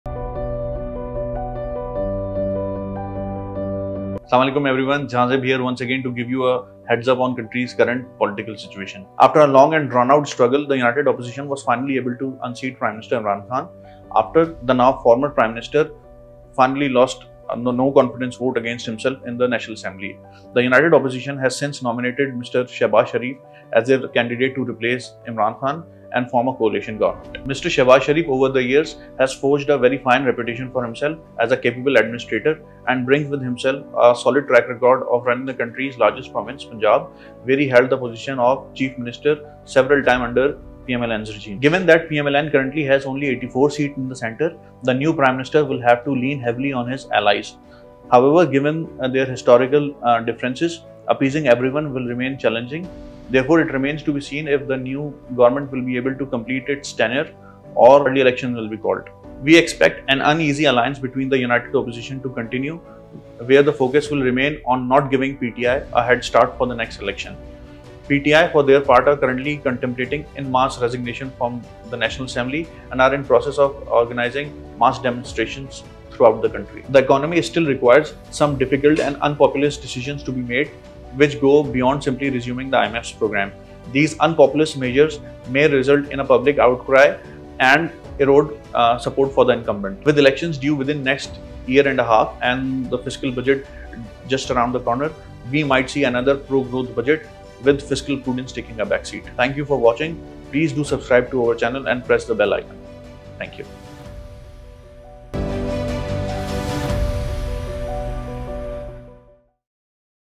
Opposition finally manages to unseat PM
Jehanzaib Zafar, Head of Research at AKD Securities, in this video sheds light on the current political scenario in Pakistan.